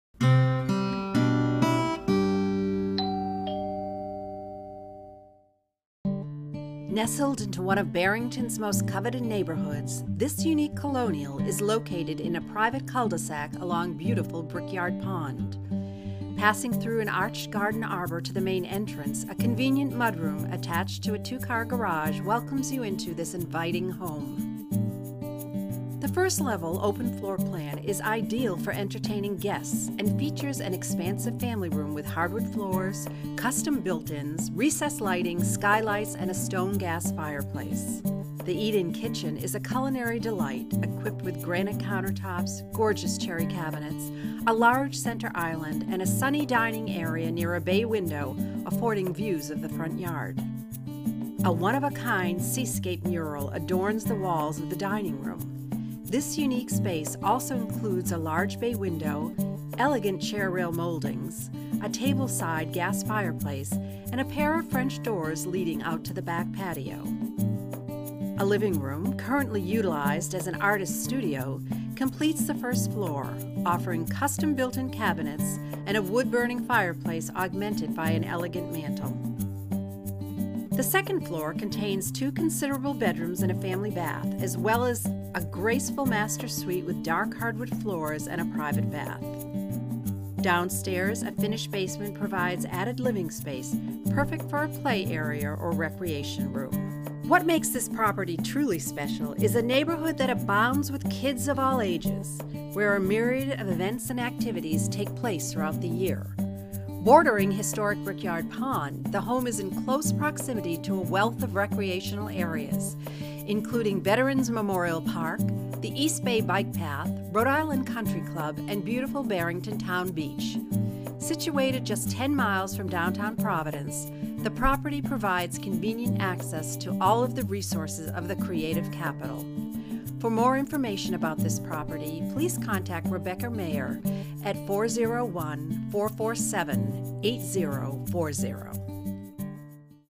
6 Overlook Road, Barrington, RI 02806
THIS NEAT-AS-A-PIN COLONIAL IS BATHED IN SUNLIGHT AND BOASTS MANY WONDERFUL IMPROVEMENTS